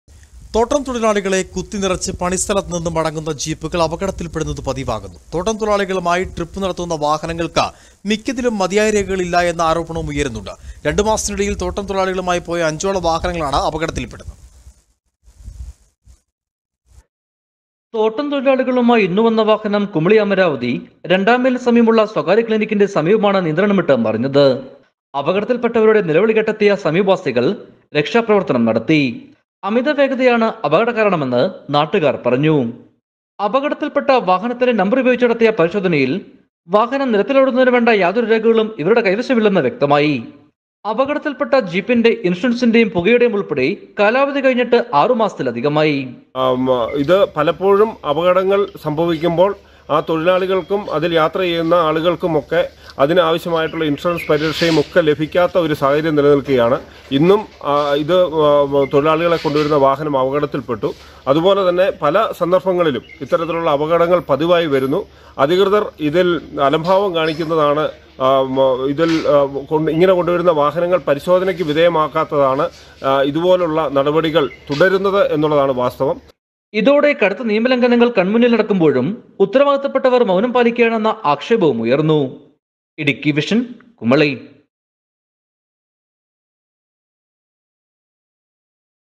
തോട്ടം തൊഴിലാളി ജീപ്പുകള്‍ അപകടത്തില്‍പ്പെടുന്നത് പതിവാകുന്നു
തോട്ടം തൊഴിലാളികളെ കുത്തിനിറച്ച് പണിസ്ഥലത്ത് നിന്നും മടങ്ങുന്ന ജീപ്പുകള്‍ അപകടത്തില്‍പ്പെടുന്നത് പതിവാകുന്നു